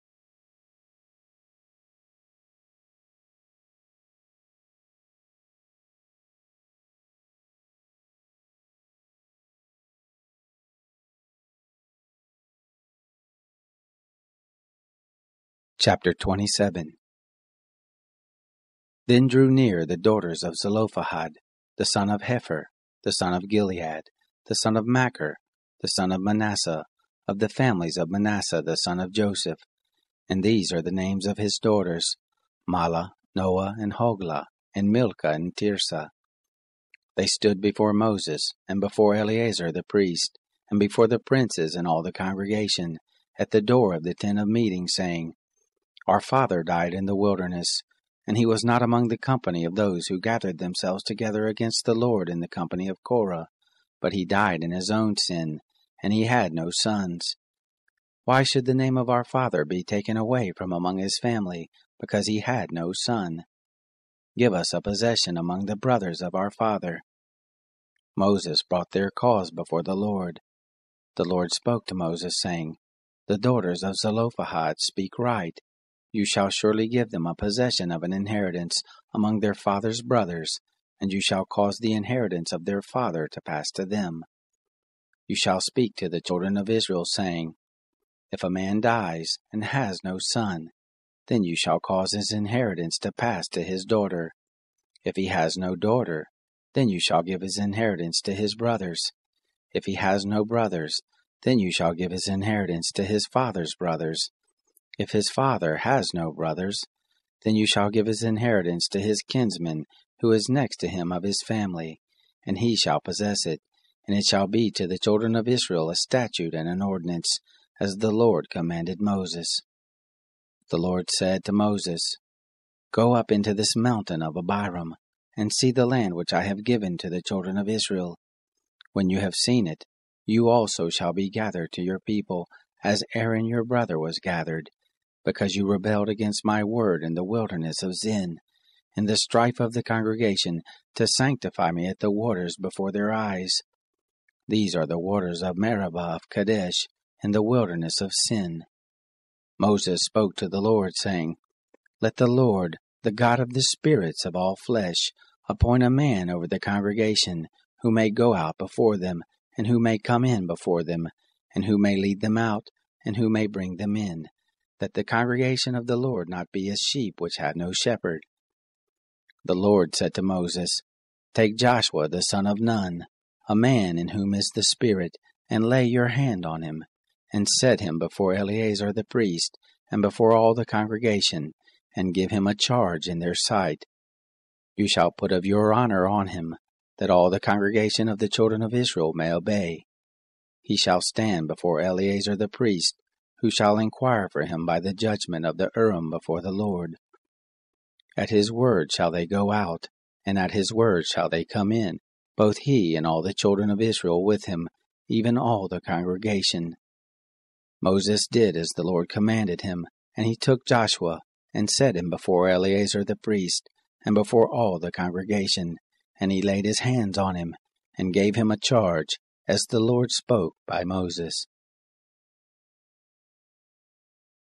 Numbers. Chapter 27
Chapter 27 of the Book of Numbers. Read from the the World English Bible version. This is a public domain version based on the 1901 American Standard Version.